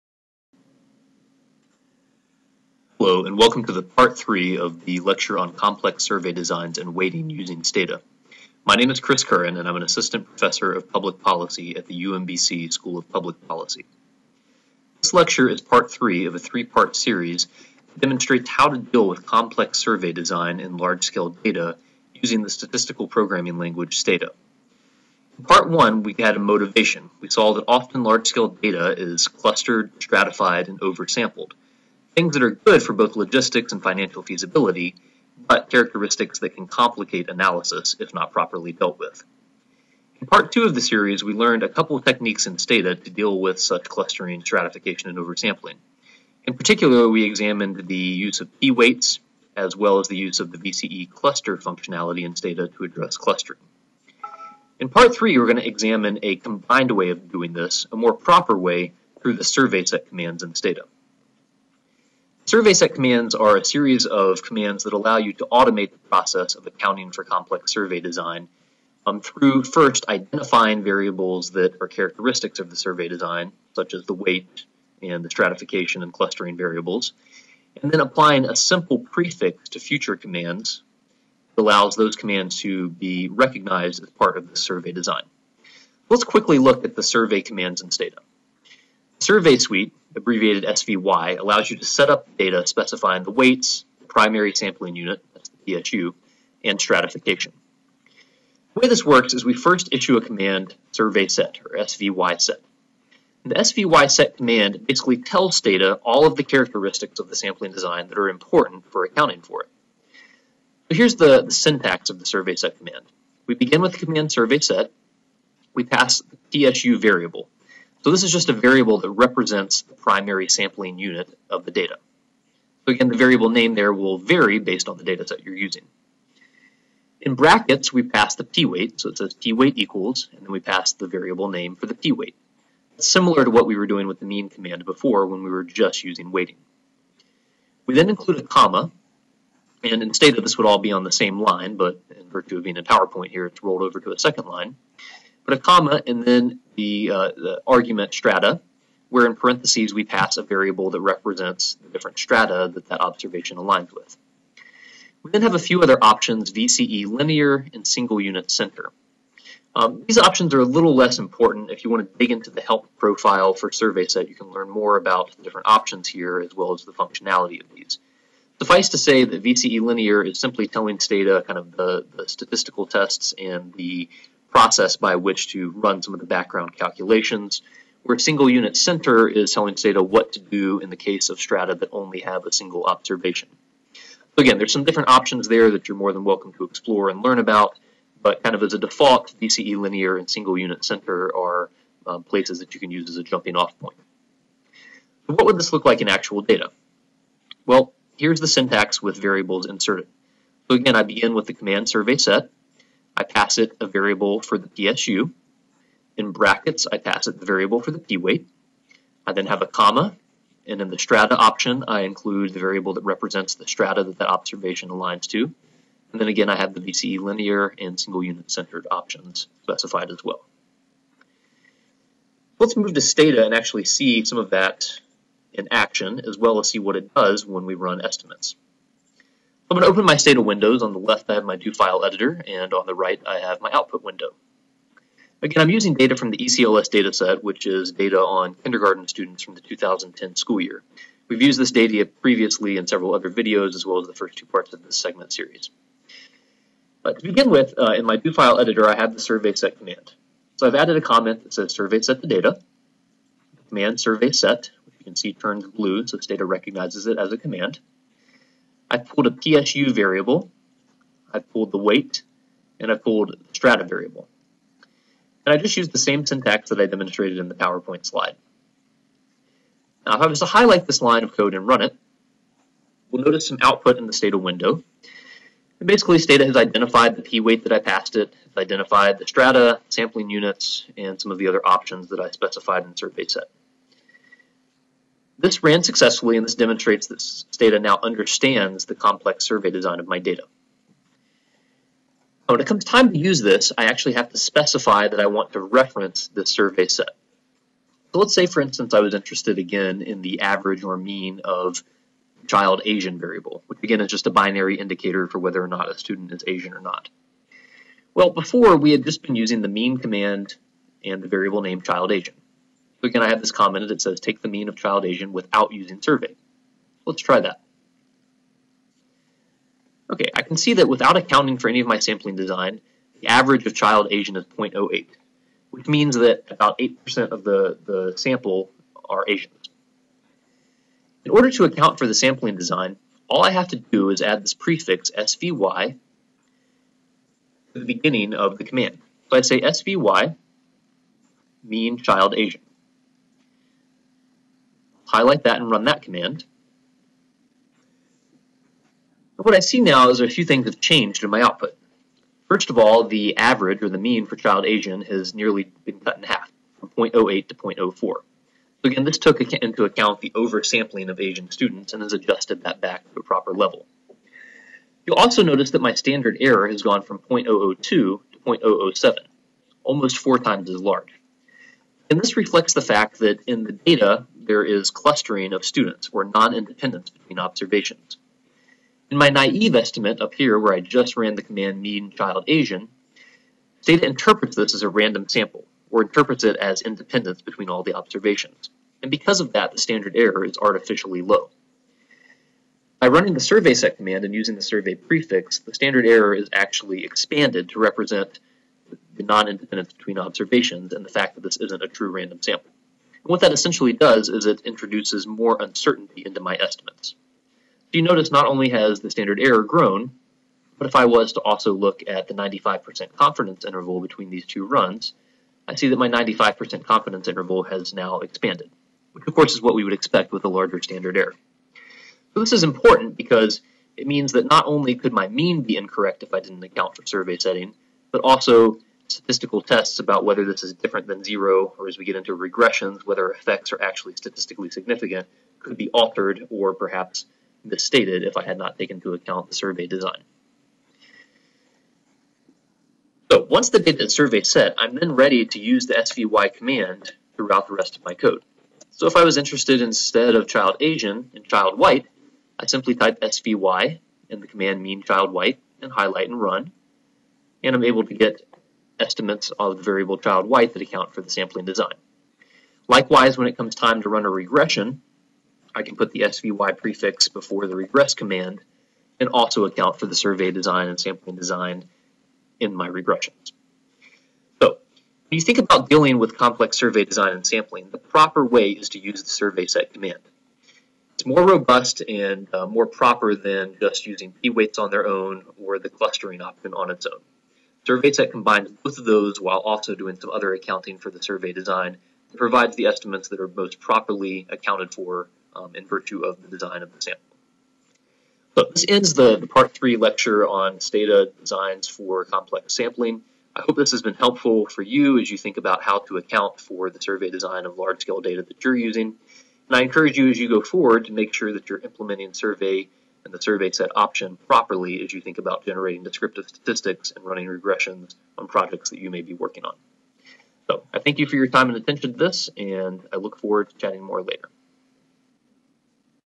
Complex Survey Designs and Weighting Using Stata: Part 3
This is the third in a three part video series that addresses the use of sampling weights and ways to account for clustering in data.